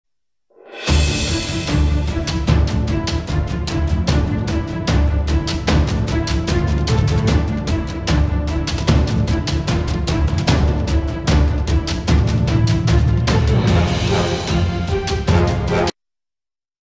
Rescue dive onboard DSV
This Short Movie produced John Alden following the Divers undertaking a 'Rescue Dive'.